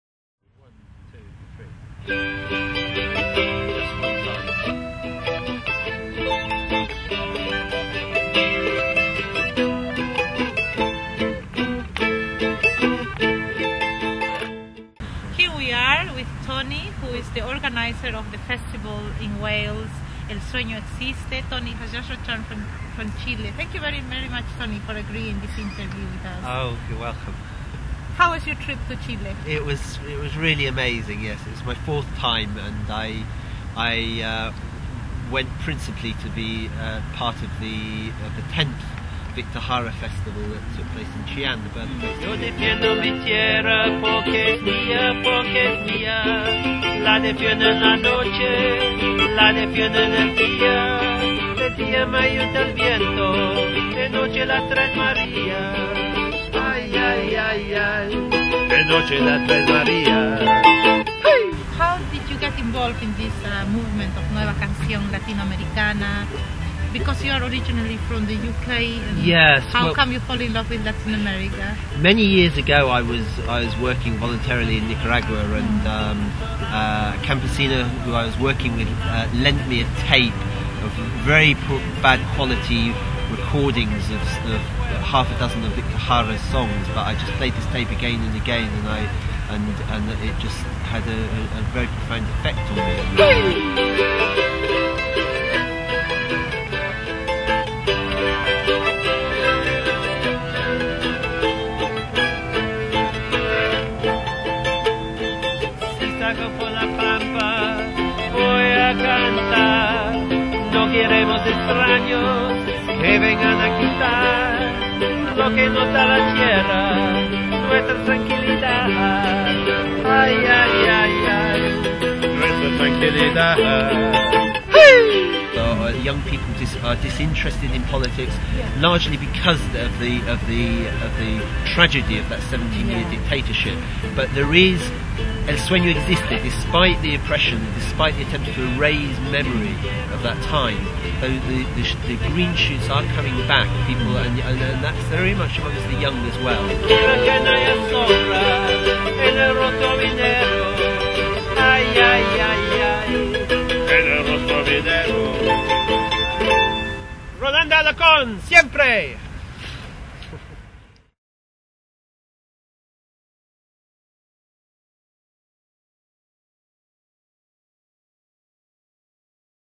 "LLAMA MUSIC" ENTREVISTA A TONY DEL SUENO EXISTE
Entrevista a Tony organizador del festival en homenaje a Victor Jara "El Sueno Existe", a su llegada a Londres de Chile 2008.  Entrevista Sofia Buchuck